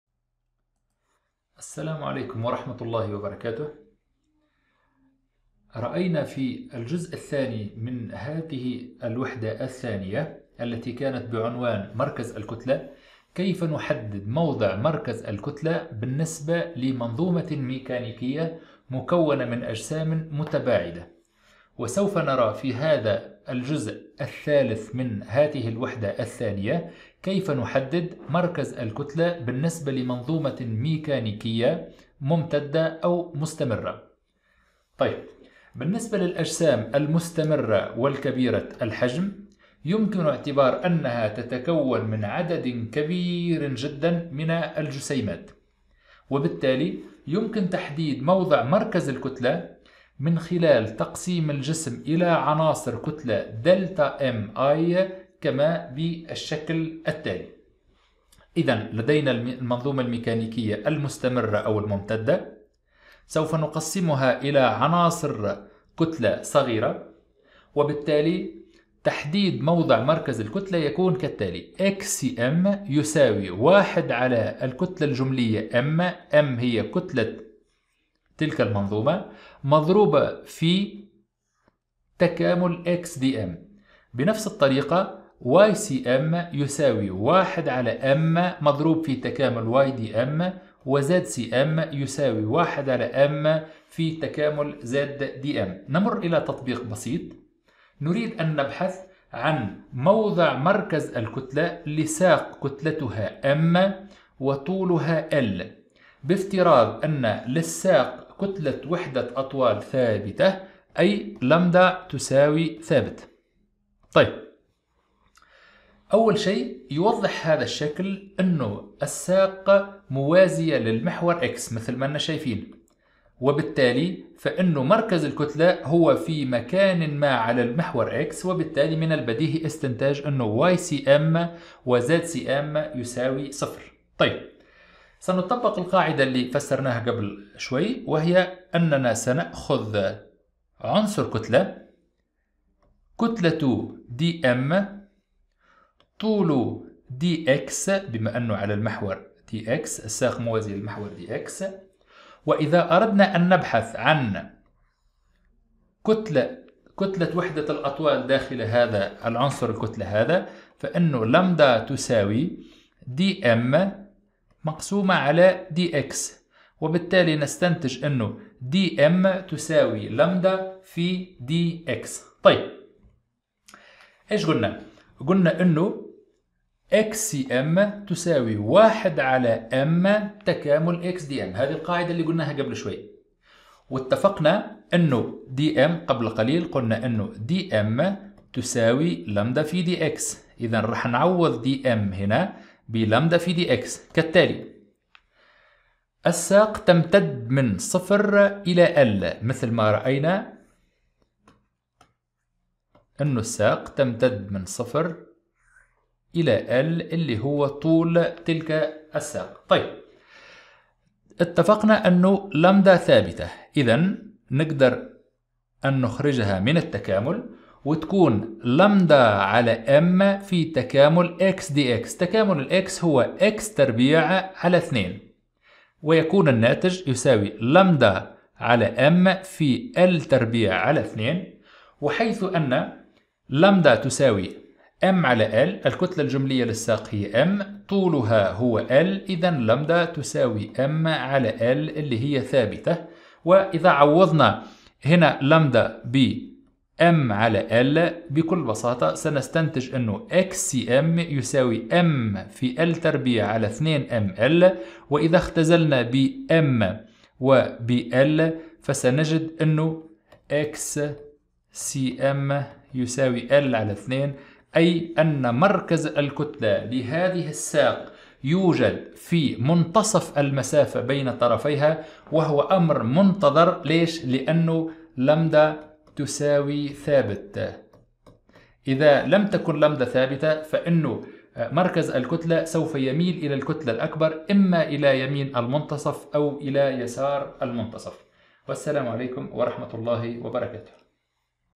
ميكانيكا 2 |2-3| مركز الكتلة | مركز الكتلة للأجسام الممتدة (مثال تطبيقي)
المقررات المفتوحة - ميكانيكا - 2
الوحدة 2 :  مركز الكتلة
الدرس 3 : مركز الكتلة للأجسام الممتدة (مثال تطبيقي)
• كيفية تحديد مركز الكتلة للأجسام الممتدة